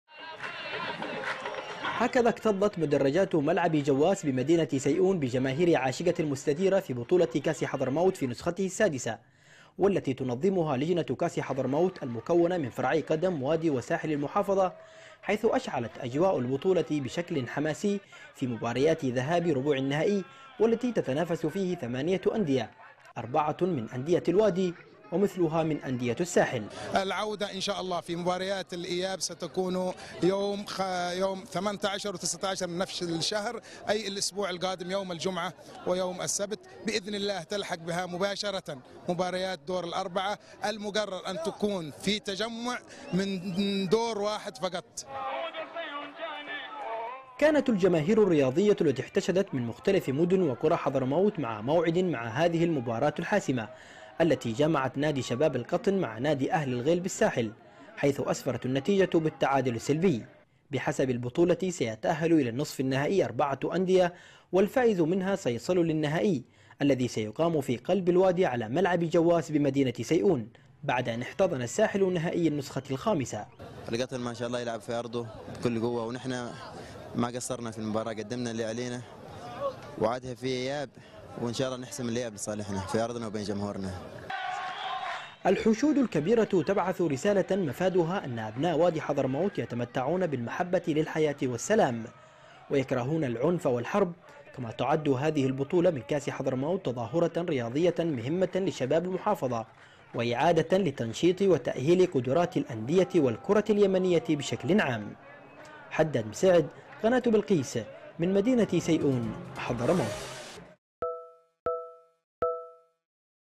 استمرار منافسة بطولة دوري كأس حضرموت وسط حضور جماهيريا كبيرا| تقرير:حداد مسيعد
تشهد بطولة دوري كأس حضرموت في نسخته السادسة تنافسا كبيرا بين الفرق المتبقية في دور الثمانية والتي تنظمها لجنة مكونة من الوادي والساحل، حيث تشهد هذه البطولة حضورا جماهيريا في مدرجات ملعب جواس بمدينة سيئون والذي سيحتضن المباراة النهائية بعد تصفيات نصف النهائي الأسبوع القادم.